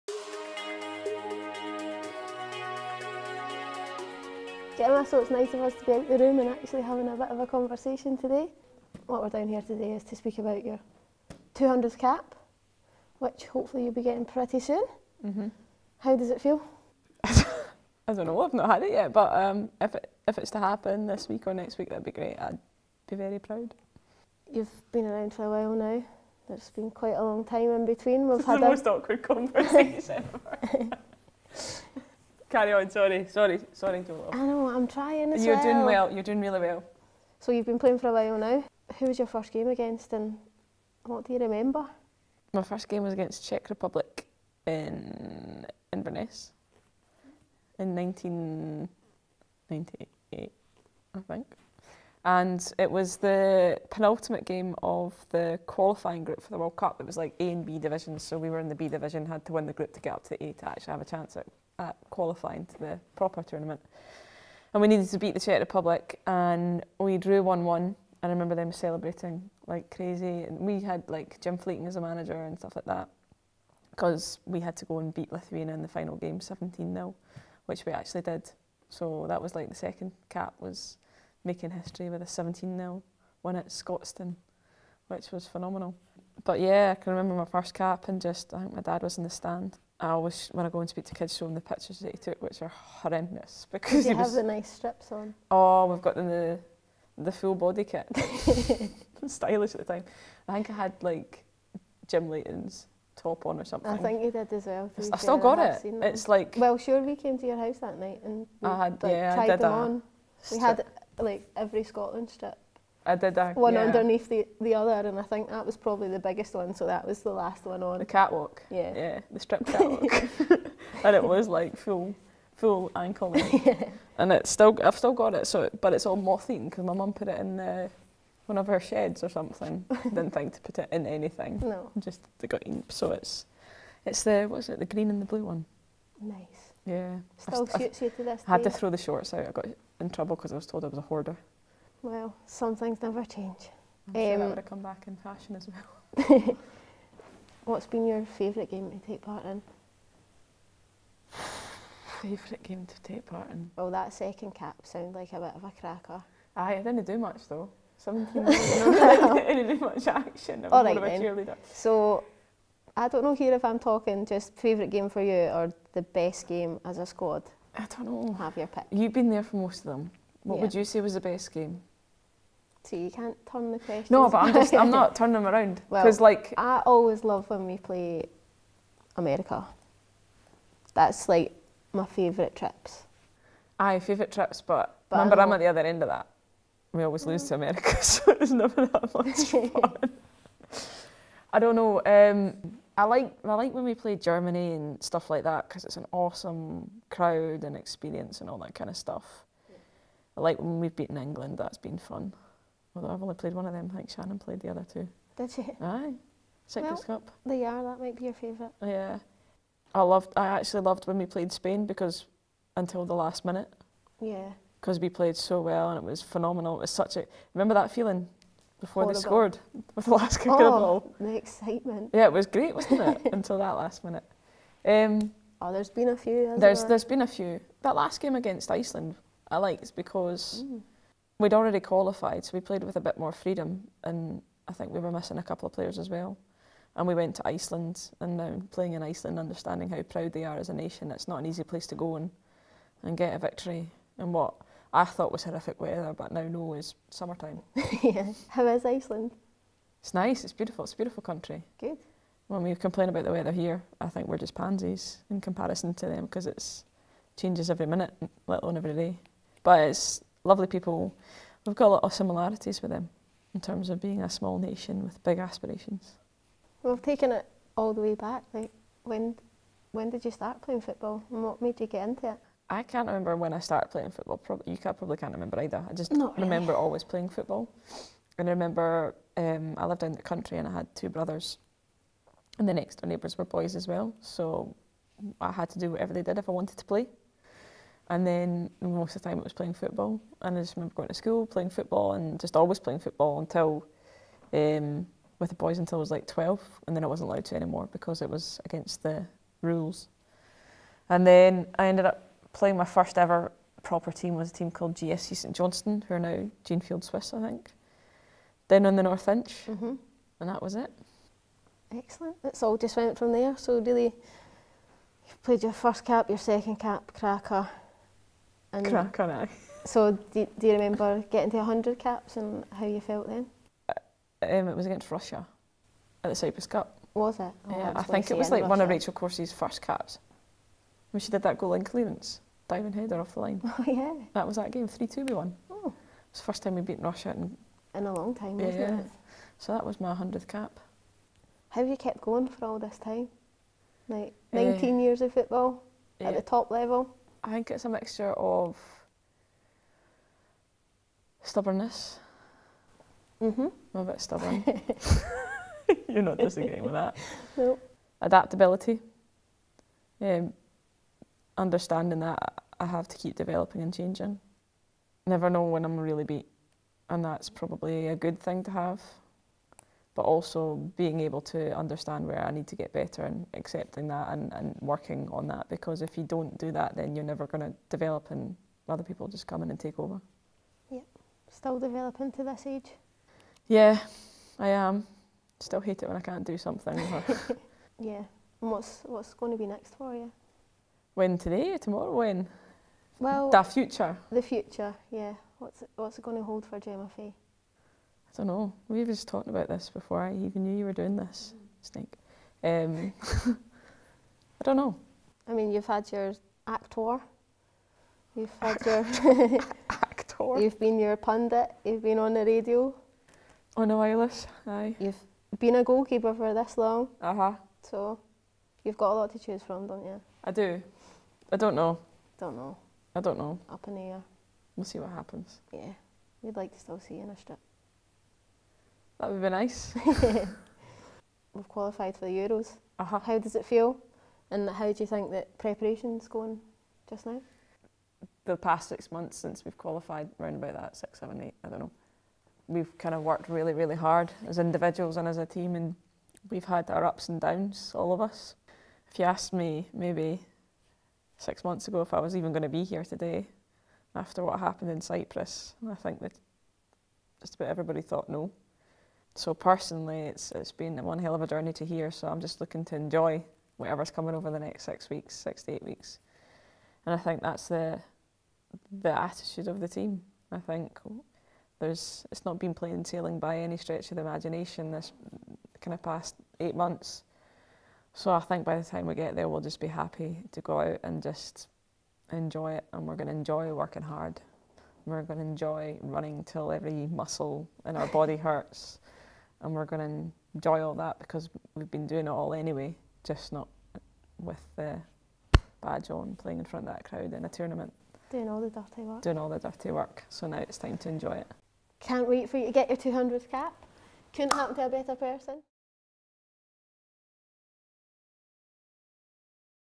Gemma Fay at 200 Caps
As she approaches her 200th appearance in a Scotland shirt, Gemma Fay sat down with long-term team mate, Jo Love, to discuss the milestone.